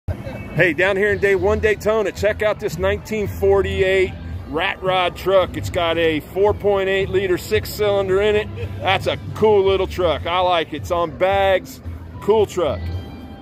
1948 Chevy Ratrod at 1DAYTONA
A 1948 Chevy Ratrod at the Cruise In at 1DAYTONA entertainment center in Daytona Beach Florida